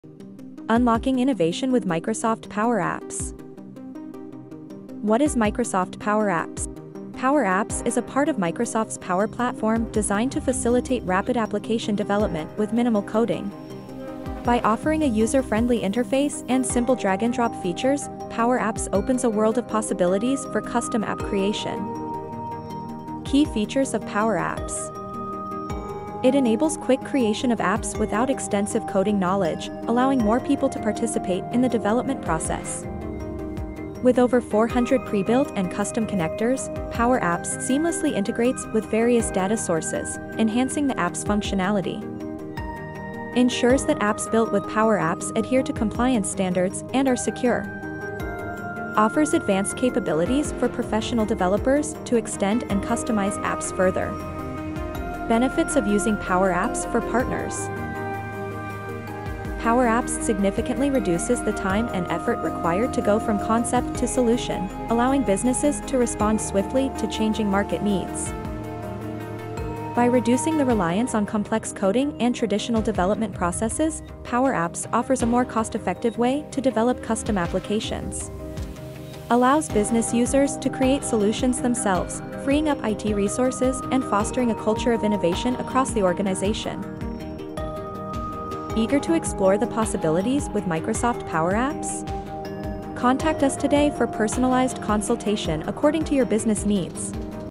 Unlocking Innovation with Microsoft Power Apps: Revolutionizing App Development
Microsoft Power Apps is revolutionizing app development with its user-friendly, low-code platform, enabling rapid creation of custom apps for various business needs.

Microsoft Power Apps is not just a tool; it’s a catalyst for transformation. It empowers businesses to solve complex problems quickly and creatively, making it an invaluable asset in the modern business toolkit. Whether it’s streamlining internal processes or creating solutions for customers, Power Apps is an ideal platform for partners looking to innovate and deliver unique value.

Eager to explore the possibilities with Microsoft Power Apps?